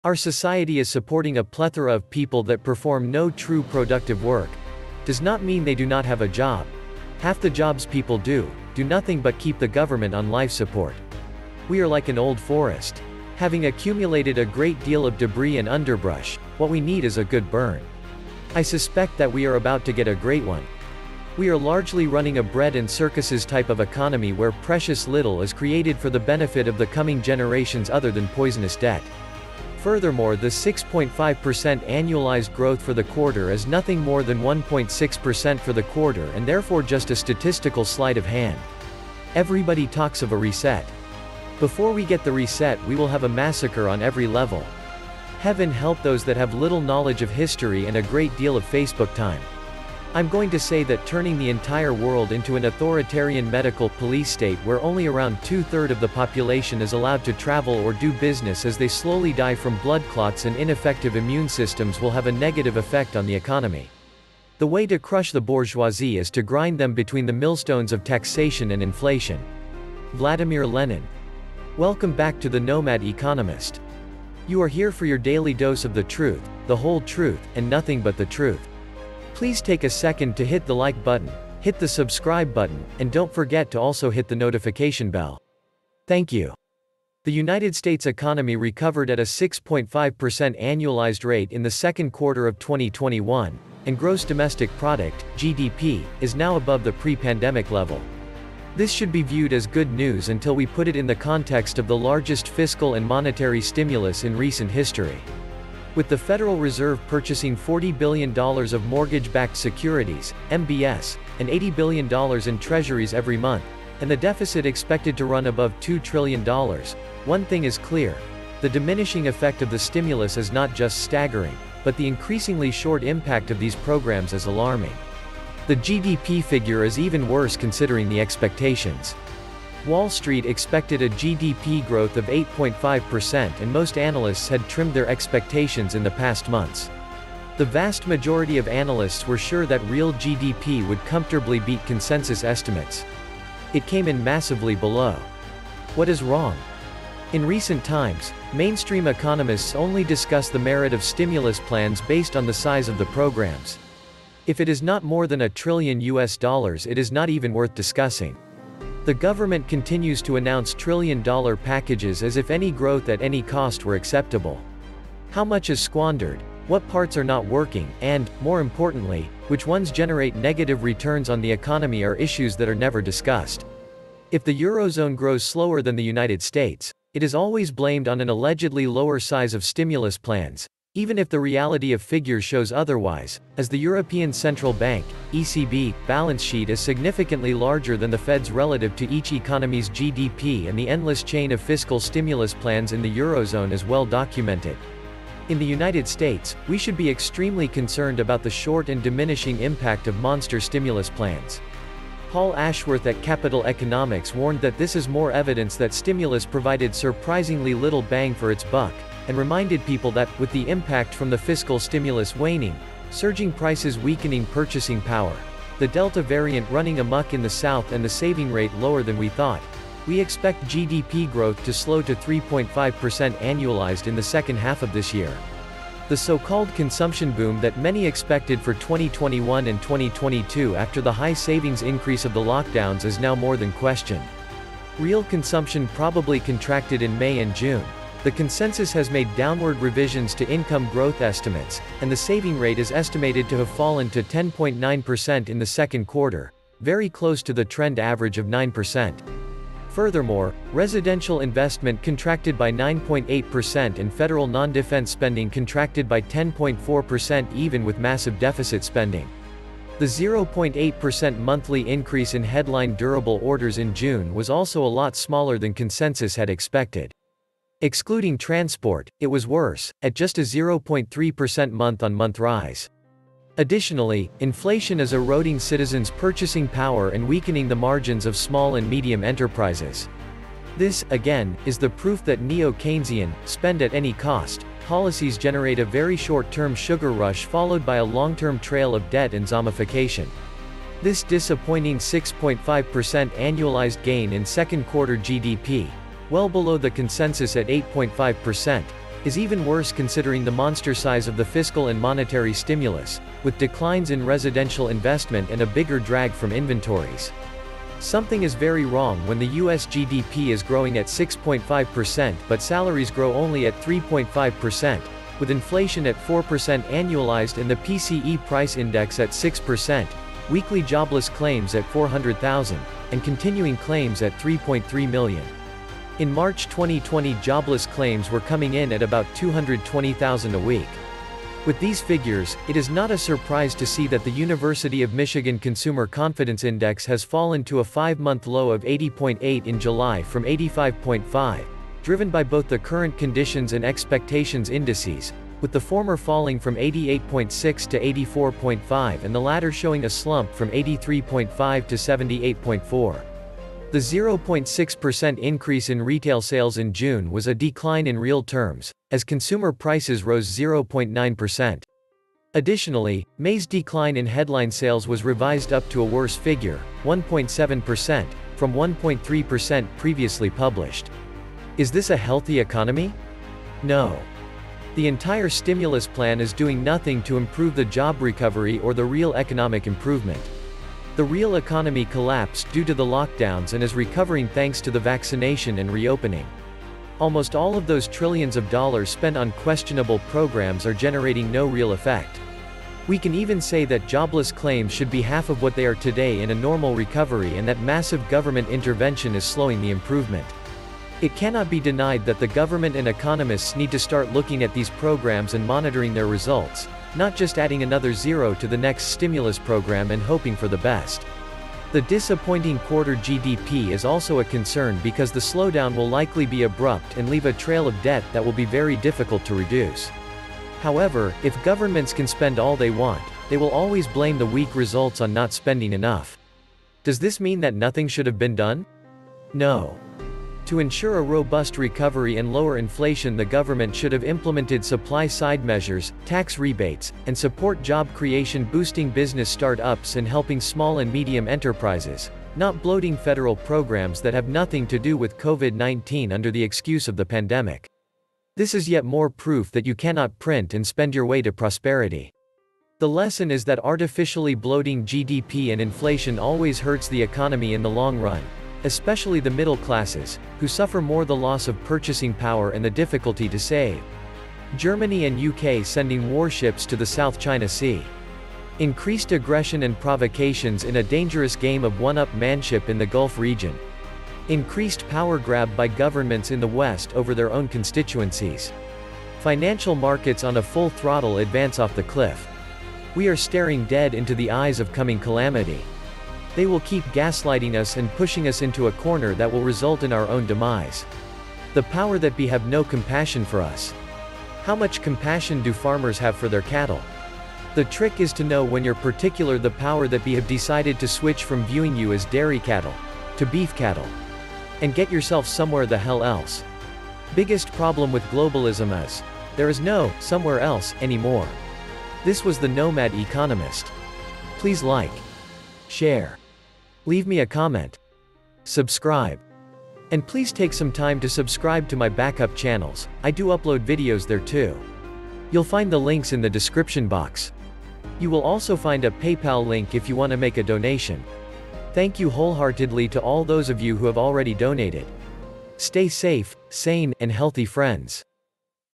Middle Class America Crushed Between Taxation & Inflation !!
Our society is supporting a plethora of people that perform no true productive work... does not mean they do not have a job... half the jobs people do, do nothing but keep the government on life support...
We are like an old forest... having accumulated a great deal of debris and underbrush what we need is a good burn...
I suspect that we are about to get a great one...
We are largely running a bread and circuses type of economy where precious little is created for the benefit of the coming generations other than poisonous debt. Furthermore the 6.5% annualized growth for the quarter is nothing more than 1.6% for the quarter and therefore just a statistical sleight of hand.